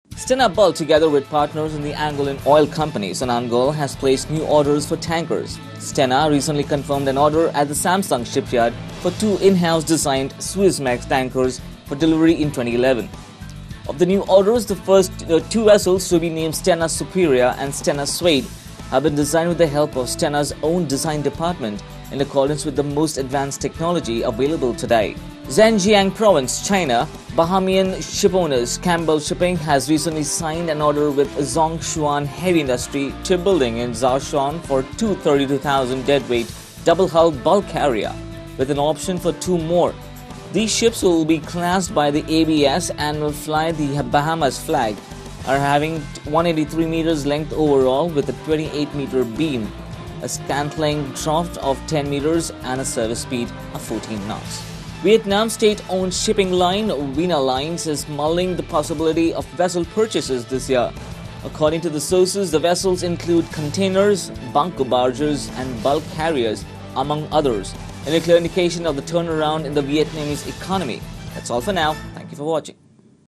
Stena Bulk places new orders for tankers.
Stena Bulk, together with partners and the Angolan oil company Sonanagol, has placed new orders for tankers. Stena recently confirmed an order at Samsung Shipyard for two in-house designed Suezmax tankers for delivery in 2011. Zhejiang Province, China: Bahamian shipowners Campbell Shipping has recently signed an order with Zhong Chuan Heavy Industry Shipbuilding in Zhoushan for two 32,000DWT double-hull bulk carriers, with an option for two more. Vietnams state-owned shipping line, Vinalines is mulling the possibility of vessel purchases this year.